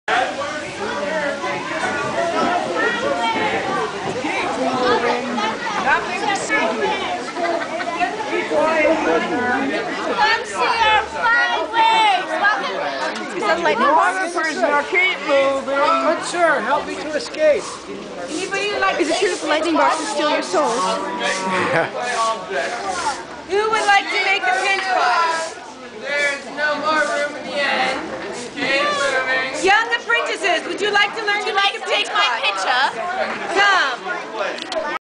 Walking through Bethlehem
Walking to through Grace Chuch of Glendora's 2009 "Walk through Bethlehem"